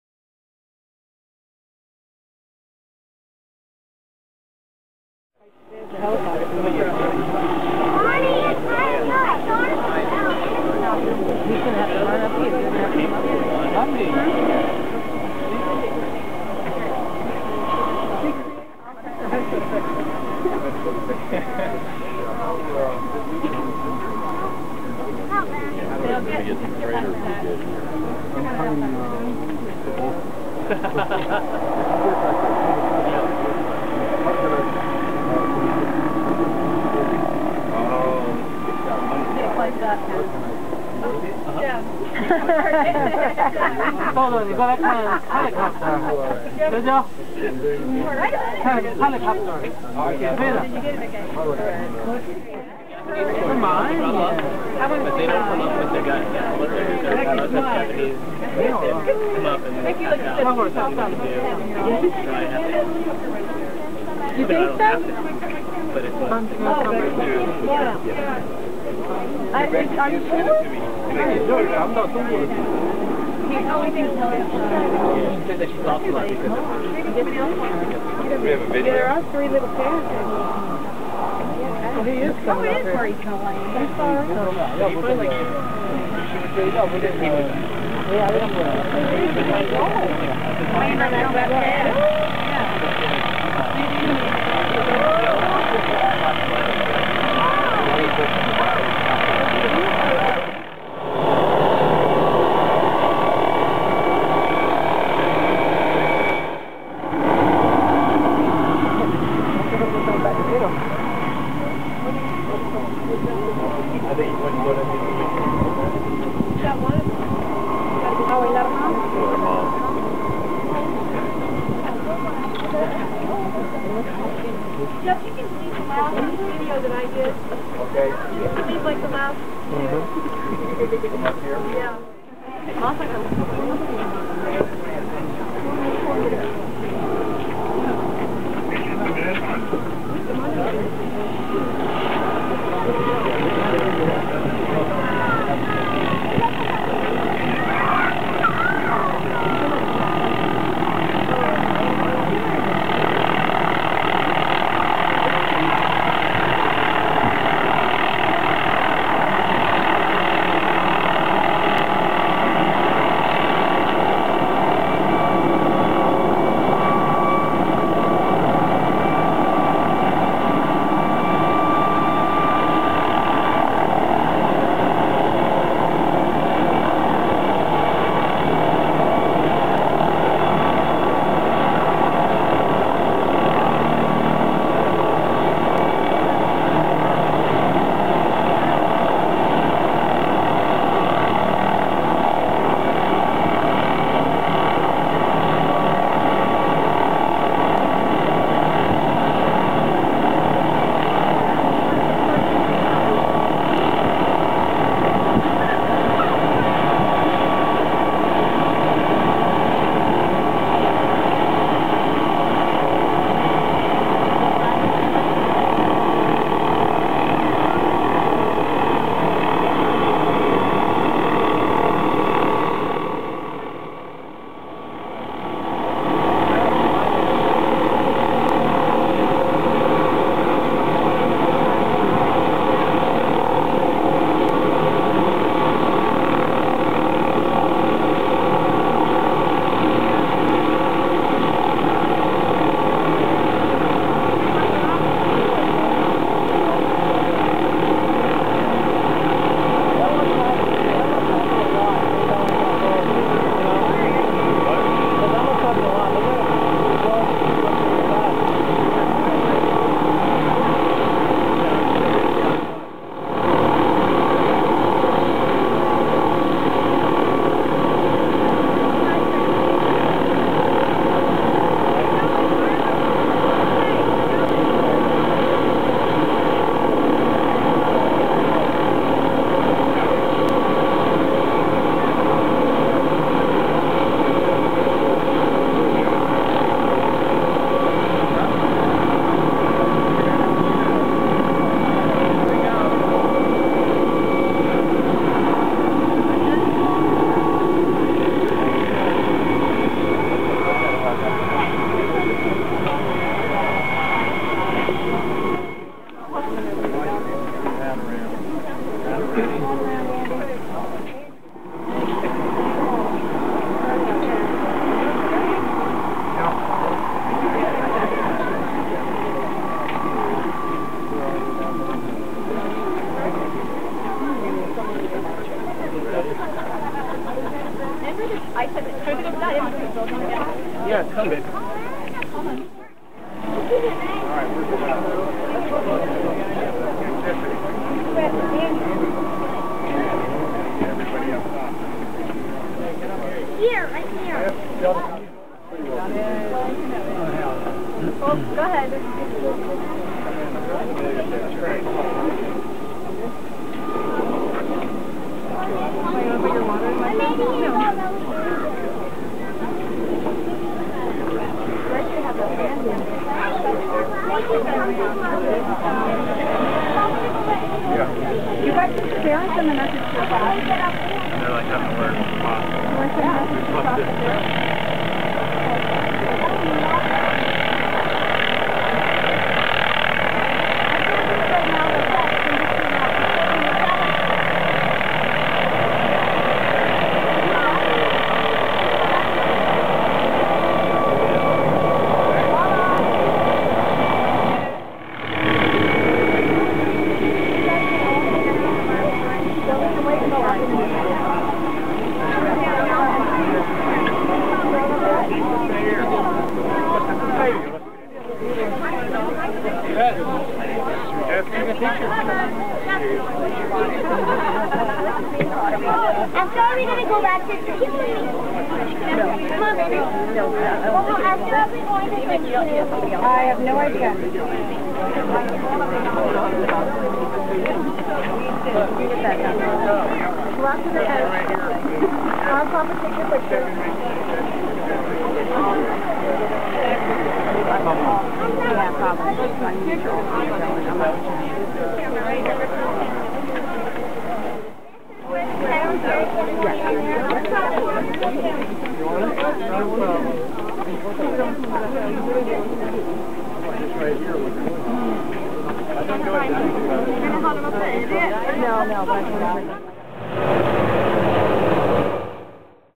【貴重映像】ハワイ_ドクターヘリ緊急出動・rescue-helicopter at Diamondhead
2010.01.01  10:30
The Honolulu Fire Department's (HFD) helicopter "Air-1"(MD-520N)@Diamondhead
お正月のダイヤモンドヘッドで急病人が発生。
ヘリが麓で救難隊員をピックアップ、そのまま一気に山頂へ向かって来ます。
とても狭い場所への神技的な着陸にも注目です。